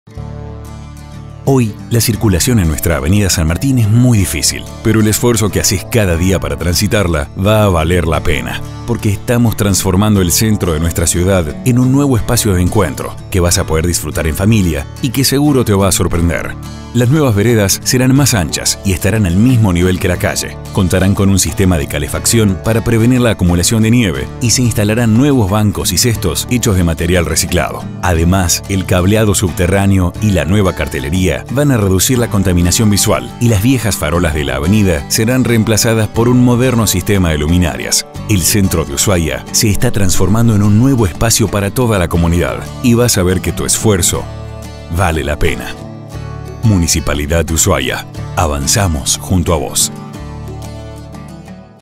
Vale la pena!
Hoy, la circulación en nuestra avenida San Martín es muy difícil, pero el esfuerzo que hacés cada día para transitarla, va a valer la pena.
Porque estamos transformando el centro de nuestra ciudad en un nuevo espacio de encuentro, que vas a poder disfrutar en familia, y que seguro te va a sorprender:
Las nuevas veredas serán más anchas y estarán al mismo nivel que la calle, contarán con un sistema de calefacción para prevenir la acumulación de nieve, y se instalarán nuevos bancos y cestos hechos de material reciclado.
Además, el cableado subterráneo y la nueva cartelería van a reducir la contaminación visual,  y las viejas farolas de la avenida serán reemplazadas por un moderno sistema de luminarias.
El centro de Ushuaia se está transformando en un nuevo espacio para toda la comunidad. Y vas a ver que tu esfuerzo, vale la pena!